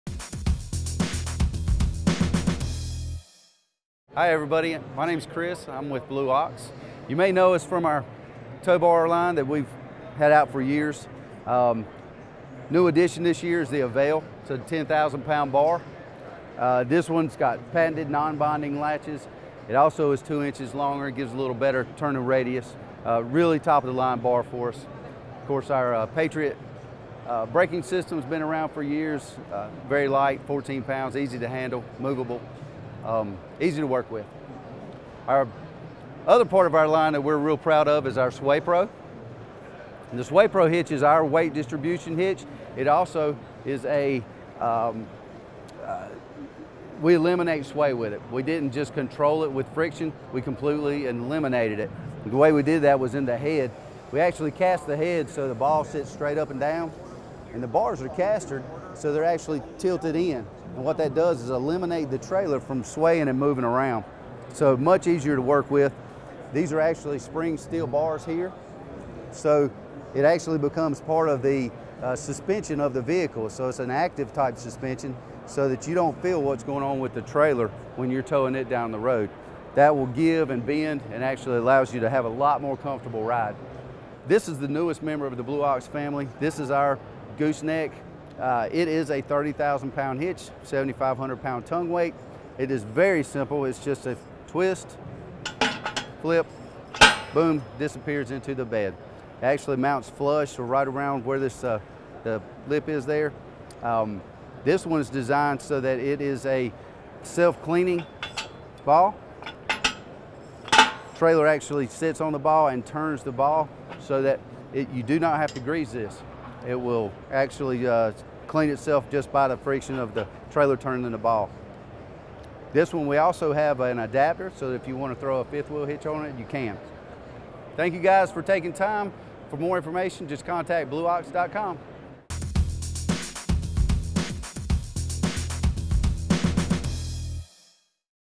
Blue Ox in Vegas
The Sway Pro hitch and Gooseneck 30,000 lb hitch are two new products Blue Ox is bringing to the market. Chris Ervin has the details.